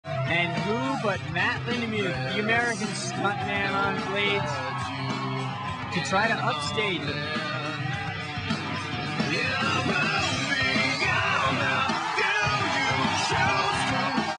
Double Lindy Matt Lindenmuths Double Backflip
Pro Rollerblader Matt Lindenmuth
Double Backflip “Double Lindy”
Aggressive Inline Vert
In competition

Enjoi!